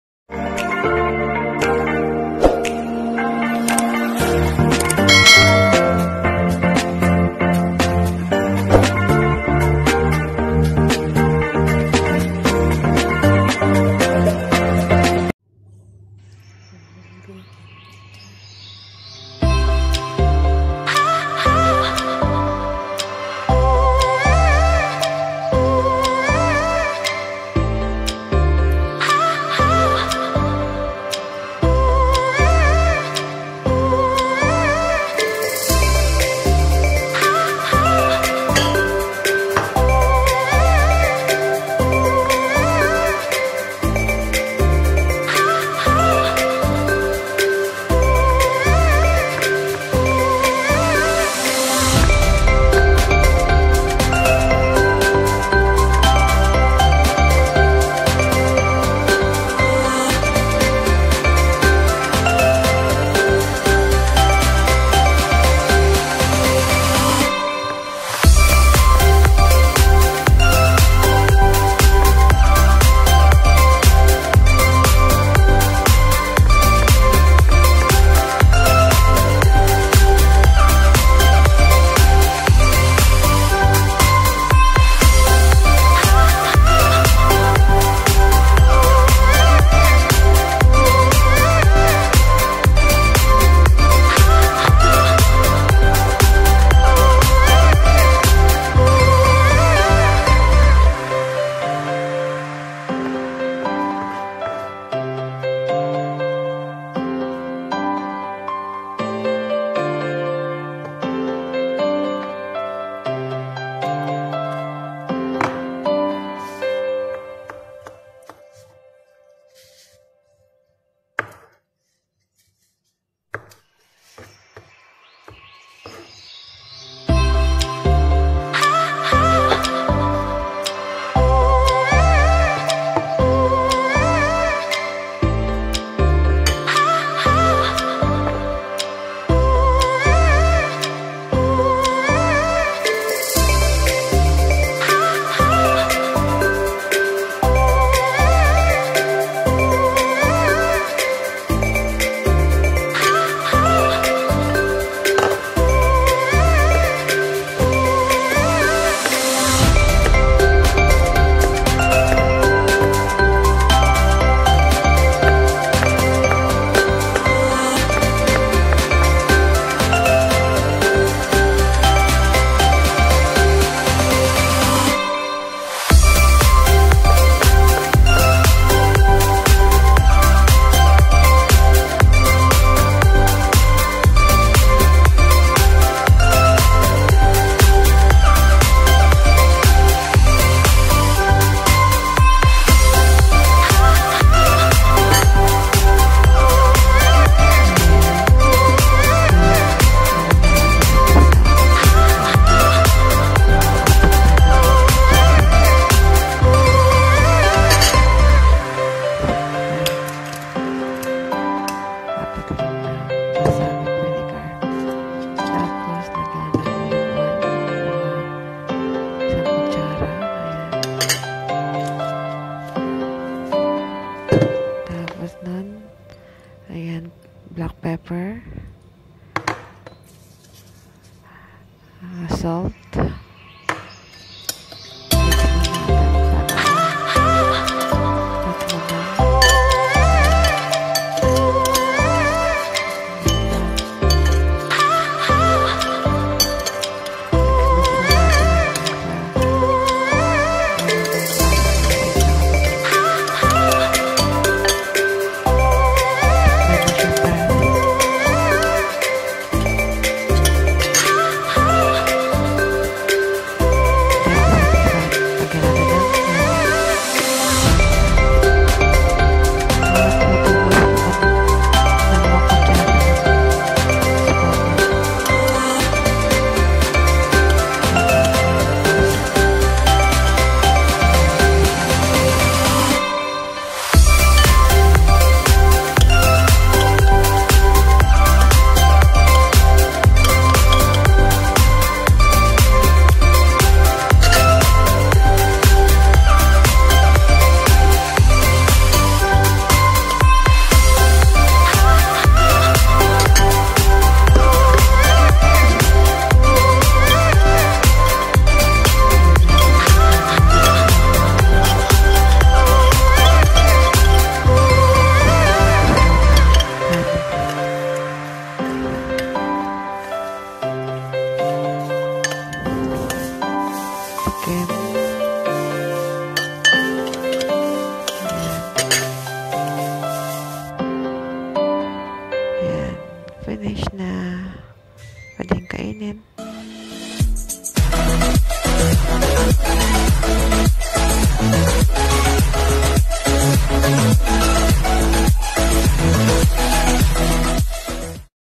betroot salad with green leaves| my own version
ang betroot salad with green leaves ay recomended sa mga taonf ng diet.